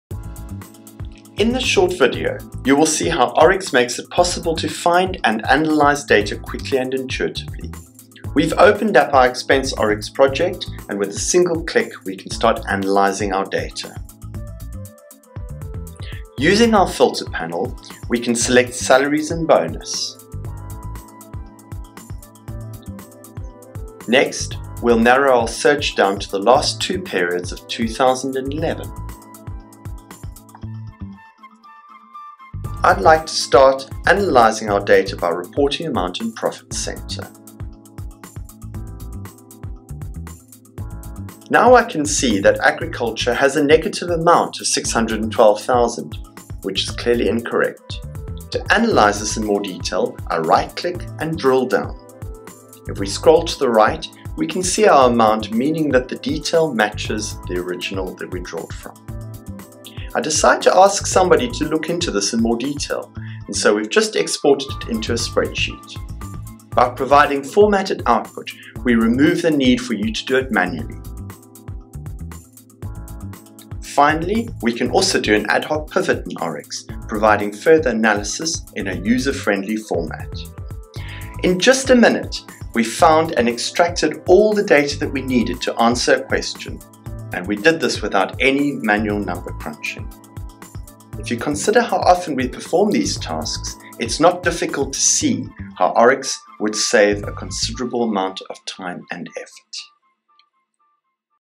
Analysing Data With ORYX | Accountagility
In this short video, you will see how ORYX makes it possible to analyse data quickly and intuitively.